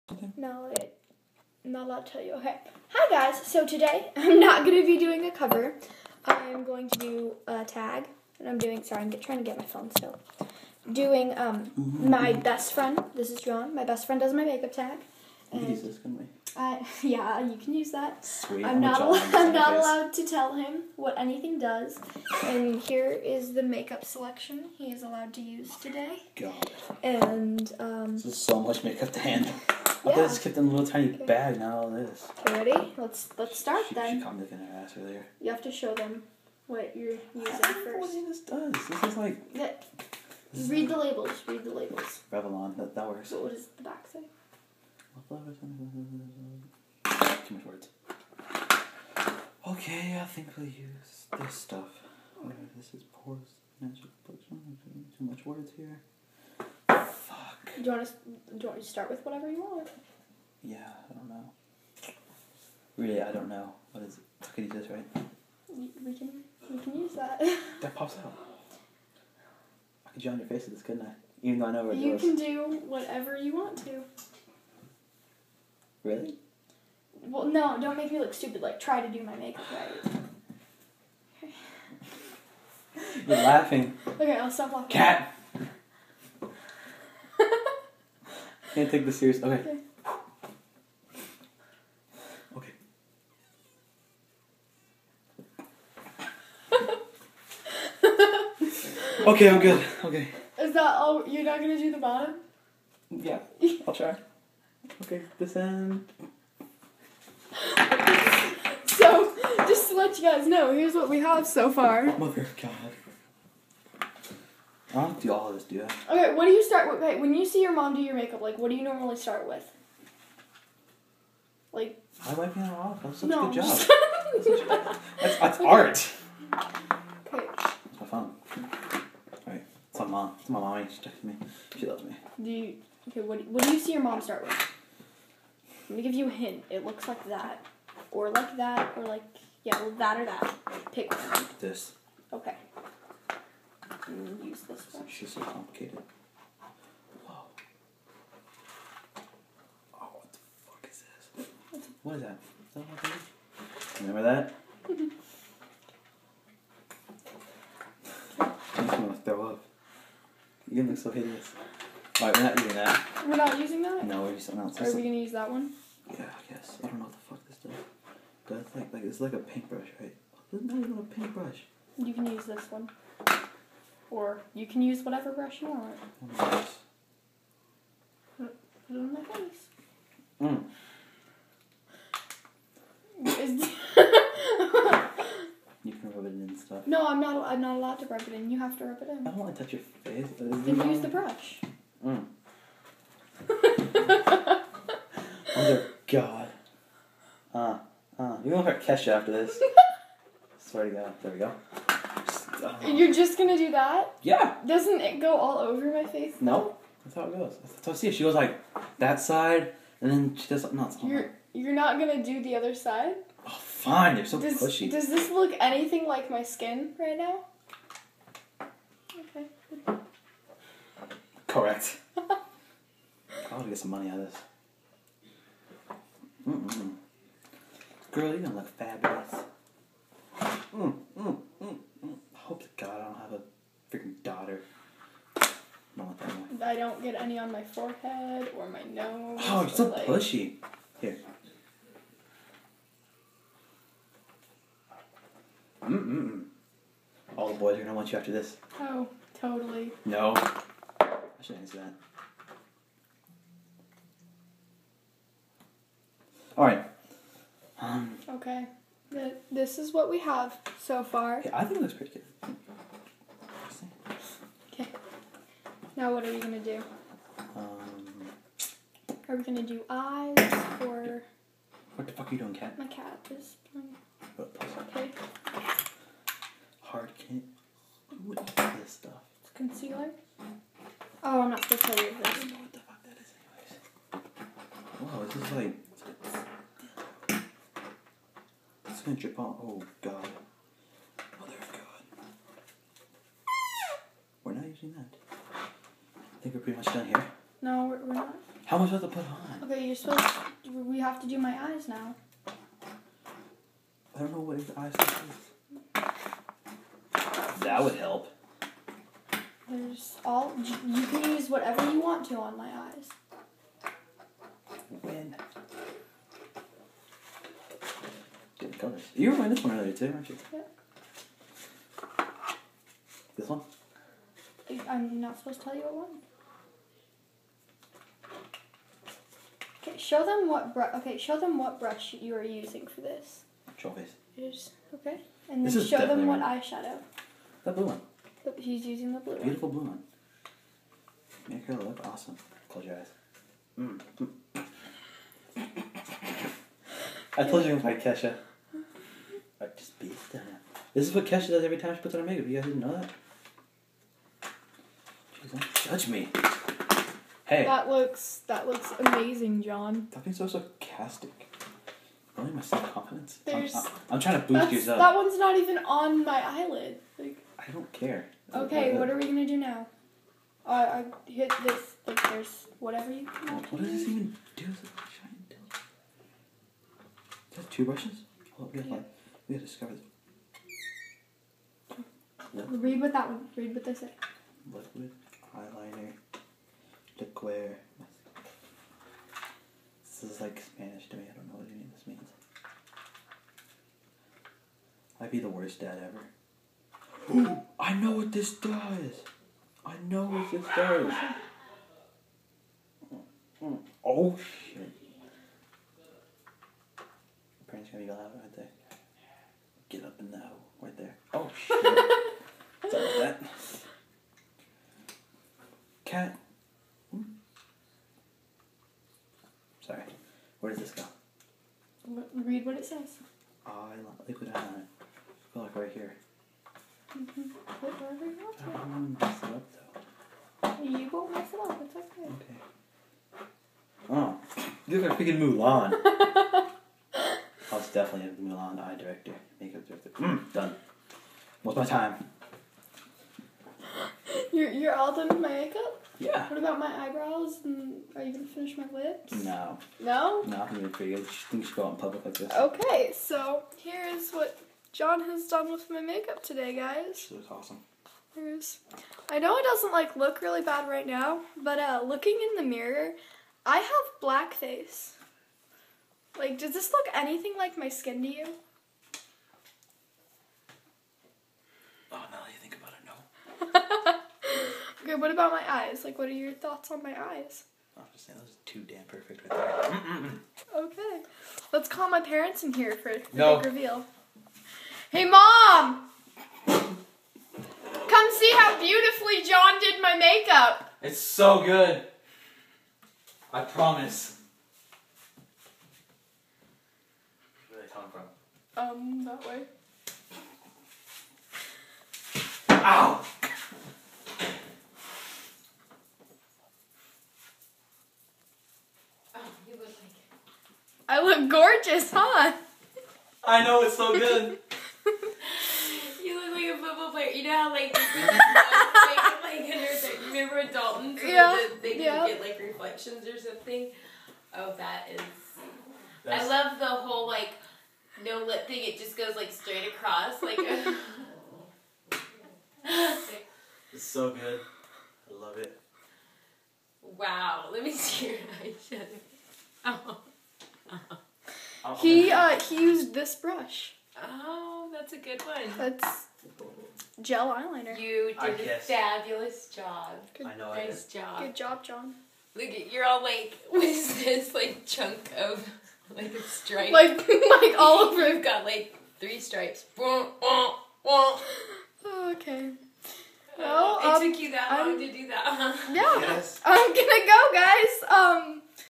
My best friend does my makeup tag
John tries to do my makeup...no were not dating, we're friends.